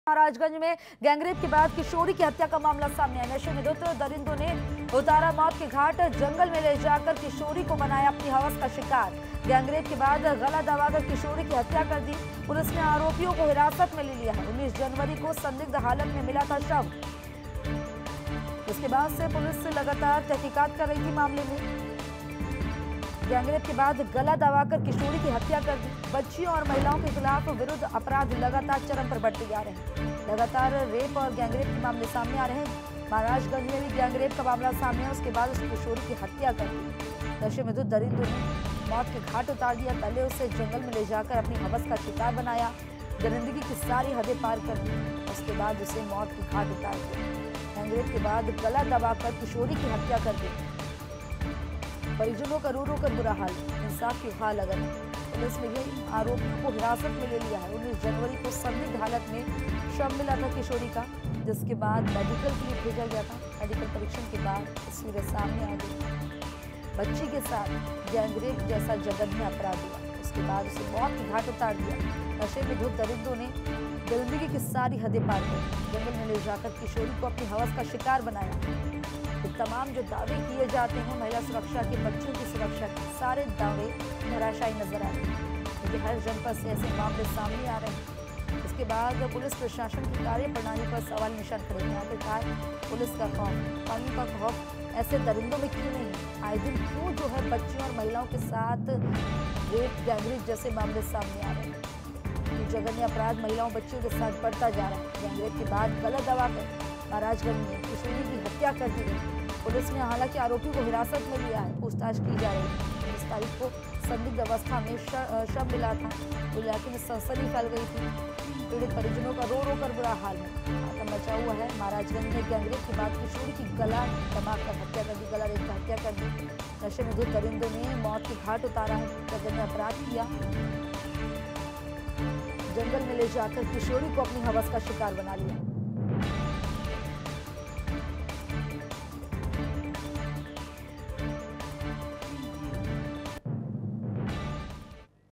महराजगंज: गैंगरेप के बाद किशोरी की हत्या, नशे में धुत दरिदों ने उतारा मौत के घाट R9TV
महराजगंज: गैंगरेप के बाद किशोरी की हत्या
नशे में धुत दरिदों ने उतारा मौत के घाट
जंगल में ले जाकर किशोरी को बनाया हवस का शिकार
गैंगरेप के बाद गला दबाकर किशोरी की हत्या
पुलिस ने आरोपियों को हिरासत में लिया

19 जनवरी को संदिग्ध हालत में मिला था शव